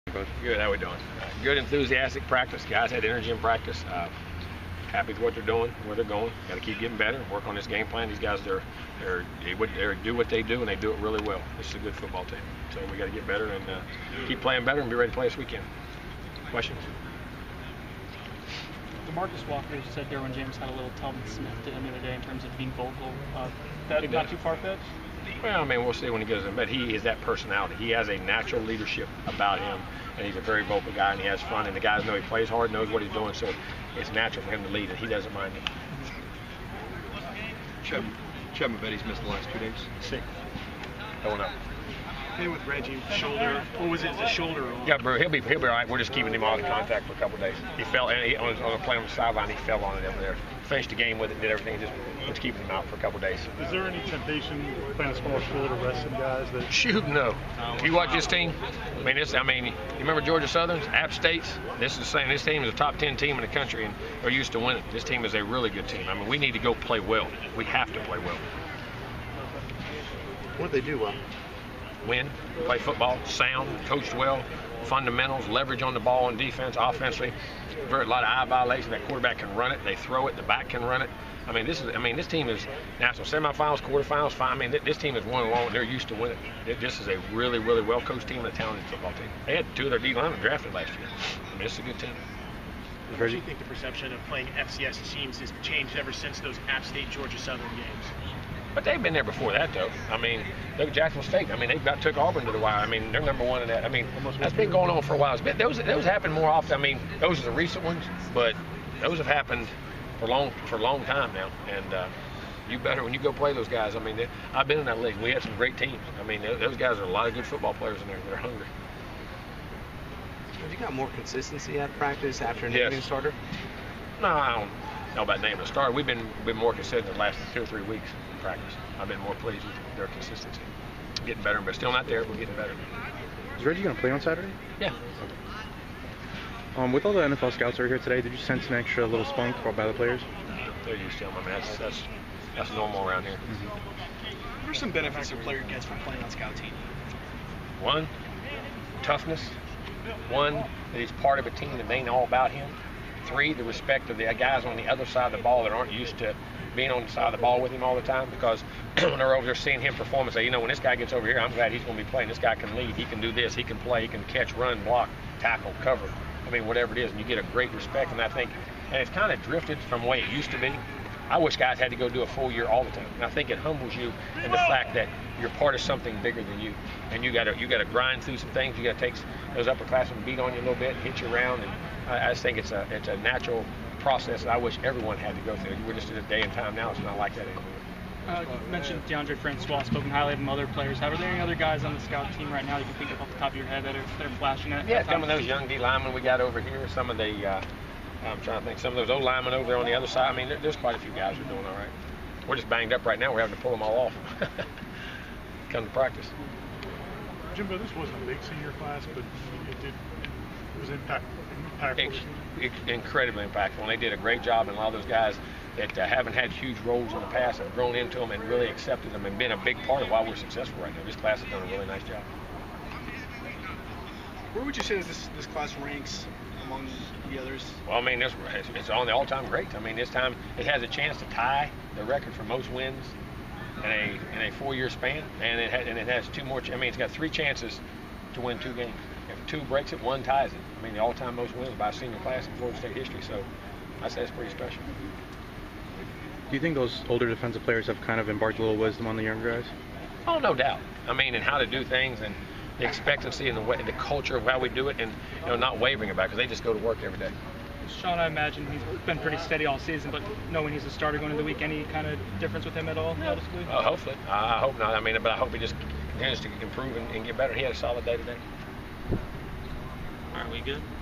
Video: Fisher talks Tuesday's practice and more
Florida State head coach Jimbo Fisher talks about Tuesday's practice and more.